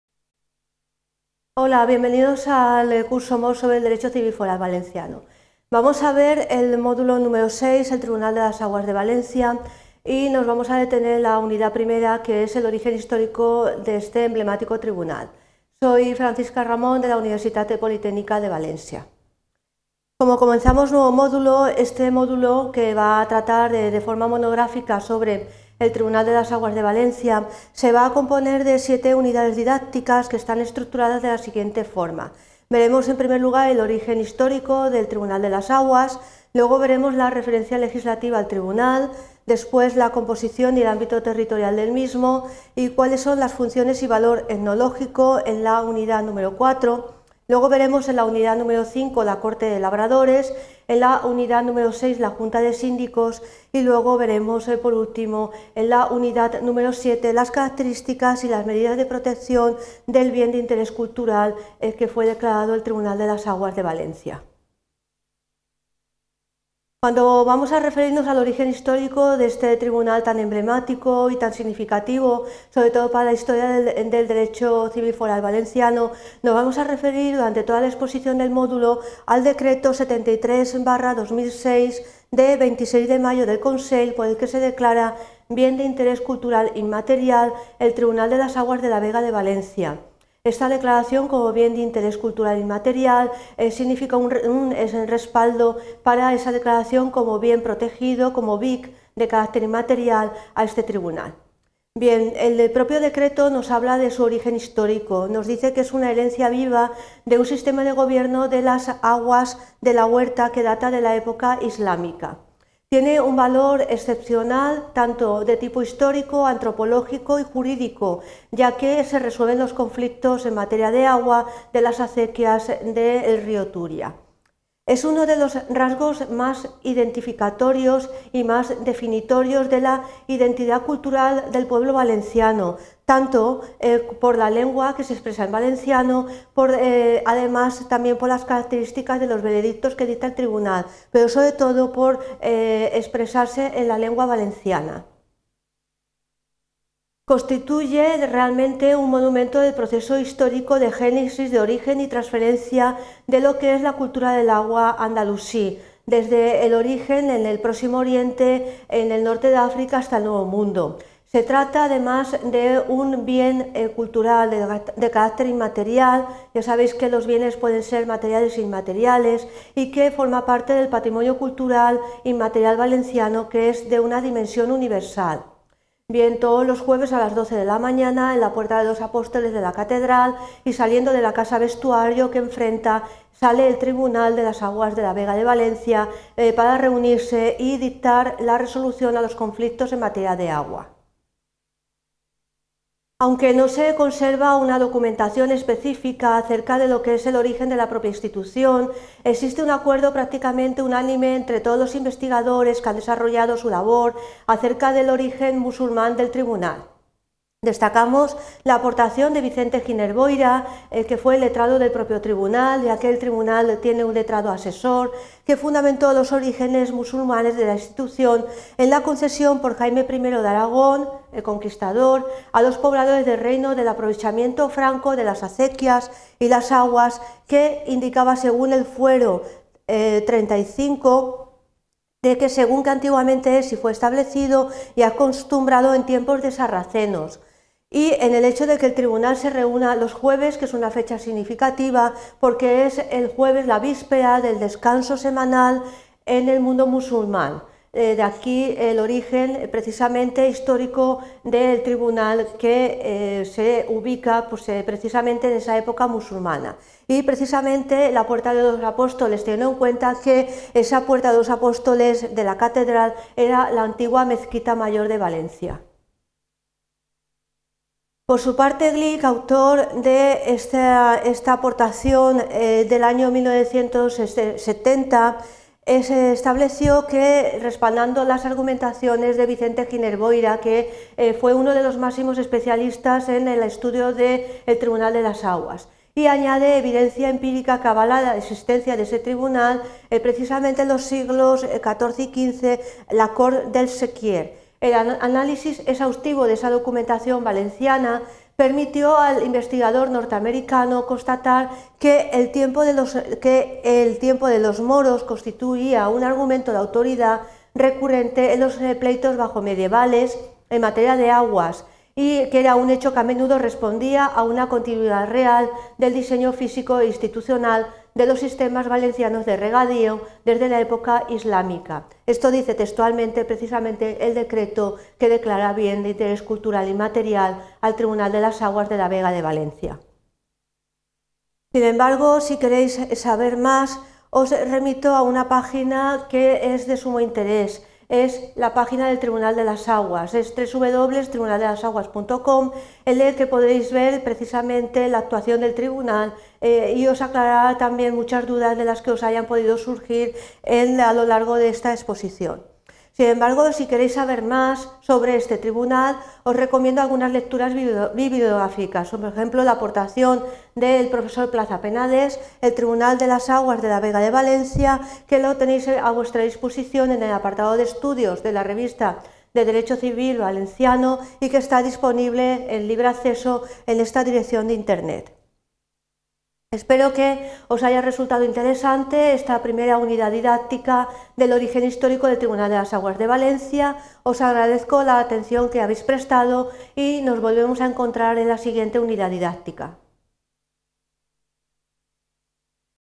Origen histórico | 37/57 | UPV
Título: Origen histórico

Descripción automática: En este video, la profesora introduce el módulo número seis del curso sobre Derecho Civil Foral centrado en el Tribunal de las Aguas. El módulo se divide en siete unidades didácticas que abarcan el origen histórico, la referencia legislativa, la composición y ámbito territorial, las funciones y el valor etnológico del Tribunal, la Corte de Labradores, la Junta de Síndicos, y las medidas de protección del bien de interés cultural.

El Tribunal de las Aguas, declarado Bien de Interés Cultural Inmaterial, es heredero del sistema de gobierno de las aguas de la huerta de la época islámica. Se resalta su importancia histórica, antropológica y jurídica, así como su papel en la resolución de conflictos relacionados con el agua del río. El decreto subraya su valor y establece que su origen data de la gestión del agua andalusí, manifestándose también en la lengua usada durante las sesiones.

Las reuniones del Tribunal se realizan todos los jueves a las doce del mediodía en la Puerta de los Apóstoles de la Catedral. Aunque falten registros específicos sobre la creación de la institución, existe consenso sobre su origen musulmán, apoyado por investigaciones y refutado por estudios que sitúan su existencia en los siglos XIV y XV.

Finalmente, para aquellos interesados en profundizar, se sugiere consultar la página web del Tribunal de las Aguas y ofrece referencias bibliográficas, como el trabajo del profesor disponible en la revista de Derecho Civil.

Autor/a: Ramón Fernández Francisca

Curso: Este vídeo es el 37/57 del curso MOOC Derecho civil foral valenciano | Universitat Politècnica de València (UPV).